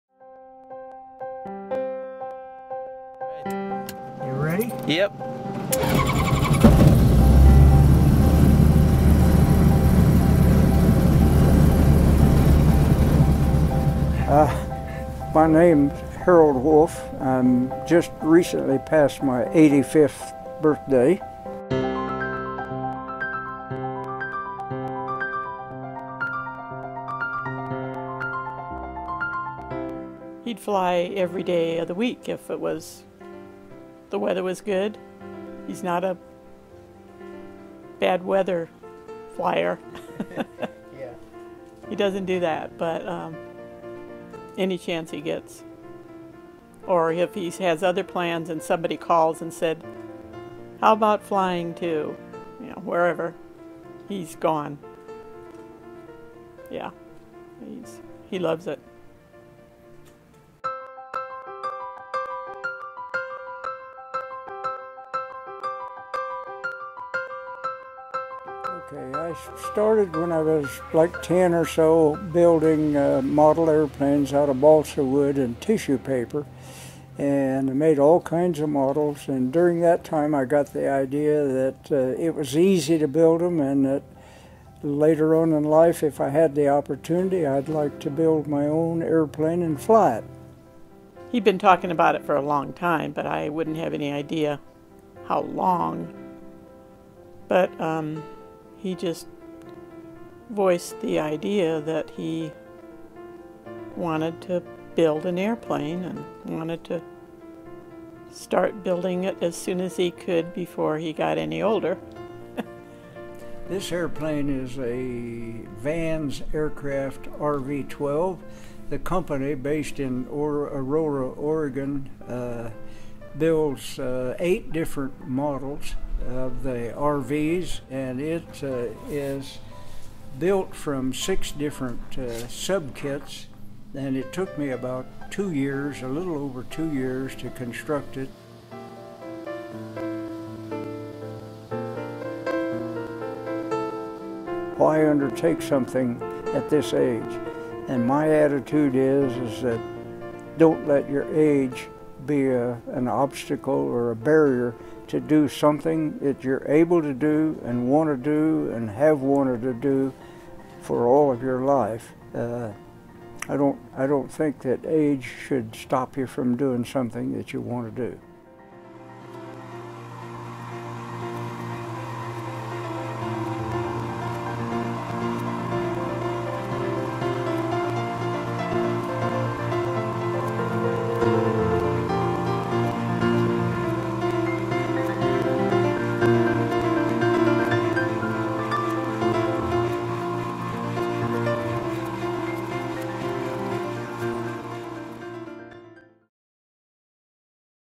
Live to Fly Short Film
A short Documentary about my Grandfather Harold Wolfe, a man who doesn't let age limit him, even at 85 years old.  Harold built an Experimental Airplane over a period of 2 years called an RV-12 and continues to fly it today.